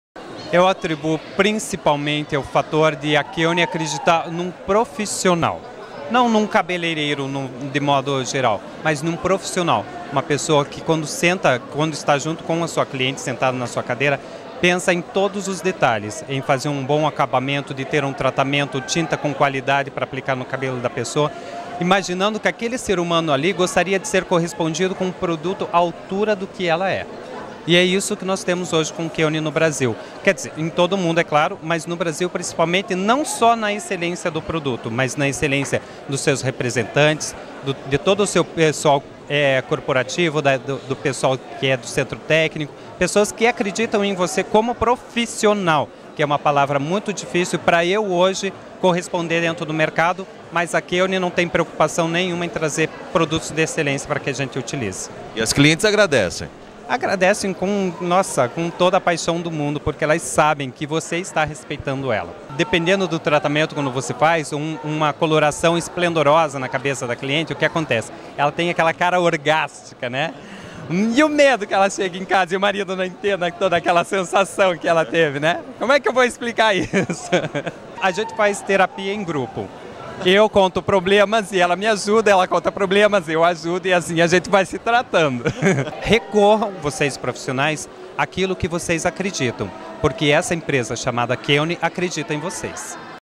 Depoimento Keune - Beto Bravo - Hair Brasil 2010.
Breve depoimento de Beto Bravo sobre a Keune Brasil.